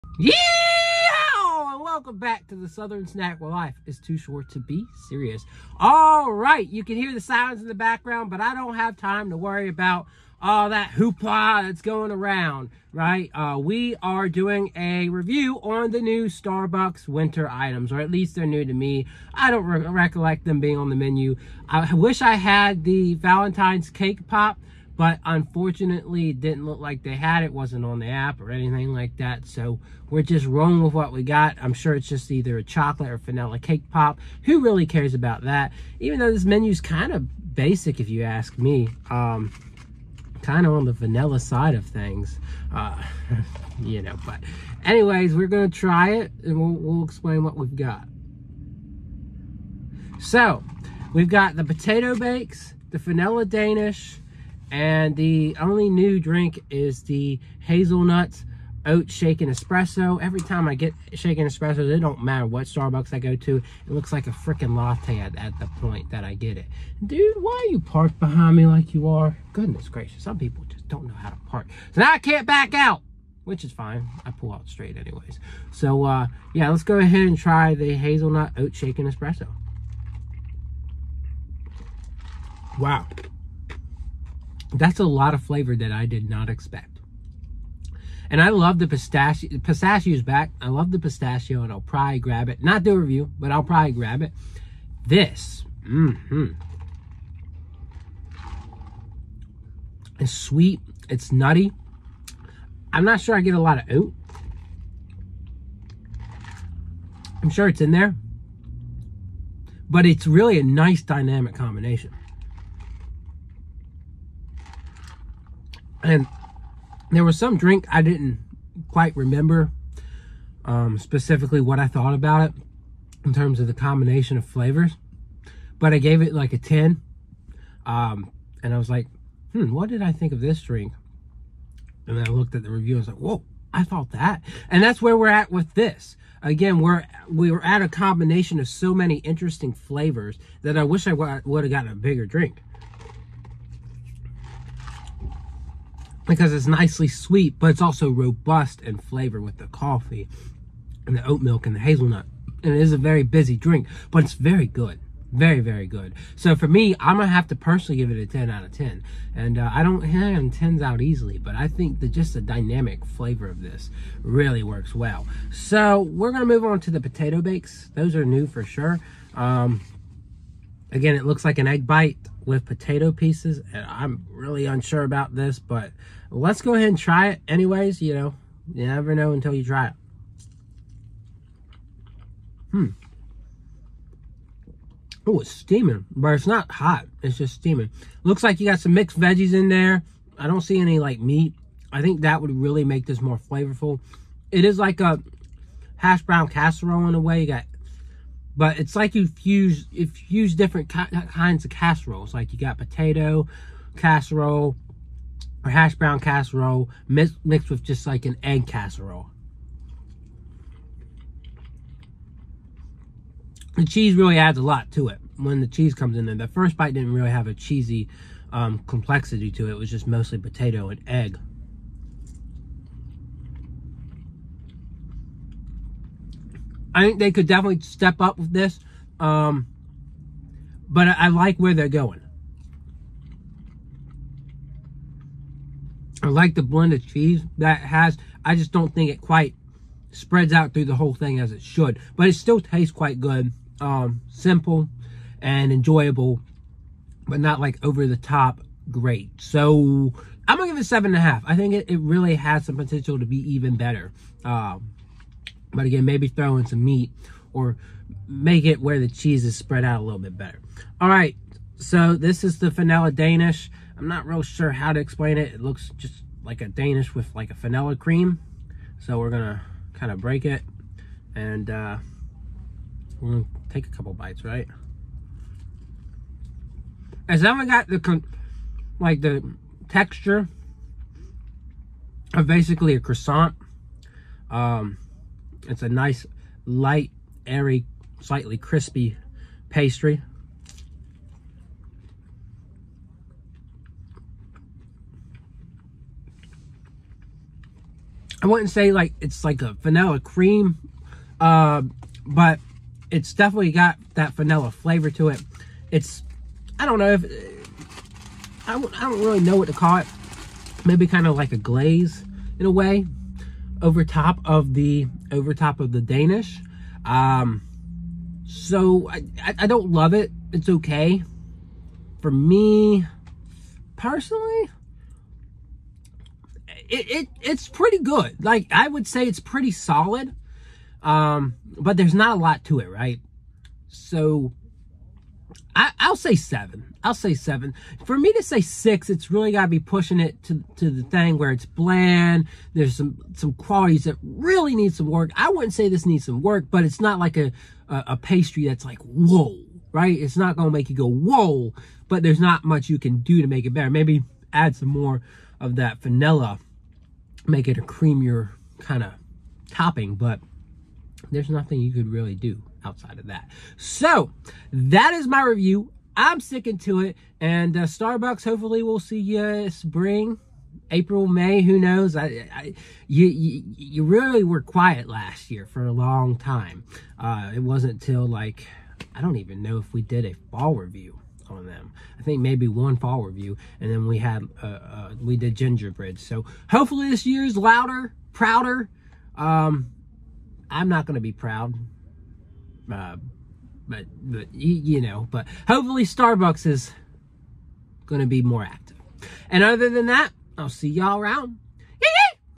Starbucks Iced Shaken Hazelnut Oatmilk Espresso, Potato Cheddar & Chive Bakes, and Vanilla Danish
@starbucks has released its winter menu for 2024!

Available as part of the coffee chain’s permanent menu, the new Iced Shaken Hazelnut Oatmilk Espresso features Starbucks Blonde Espresso and hazelnut syrup shaken together and topped with creamy plant-based oatmilk.

In addition to the new beverage, fan-favorite pistachio drinks are back for a limited time, with the Pistachio Latte and the Pistachio Cream Cold Brew returning to the winter lineup.

On the food front, Starbucks is introducing new Potato, Cheddar & Chive Bakes and new Vanilla Bean Custard Danish.

Potato, Cheddar & Chive Bakes combine cage-free eggs, diced potatoes, cheddar cheese, spinach and a touch of chives for a high-protein breakfast on-the-go.

Meanwhile, the new Vanilla Bean Custard Danish is filled with rich and velvety vanilla bean custard.

I grabbed the new espresso and new food items!